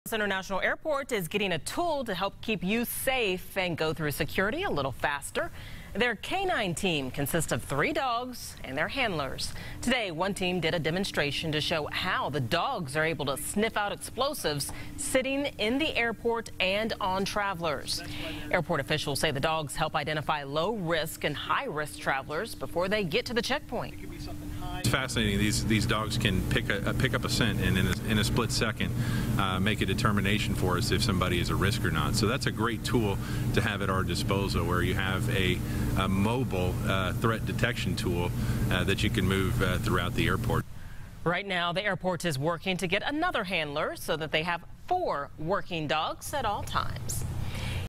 Dogs sniff out explosives at Indy airport
The Indianapolis International Airport is using a tool to help keep you safe and go through security a little faster.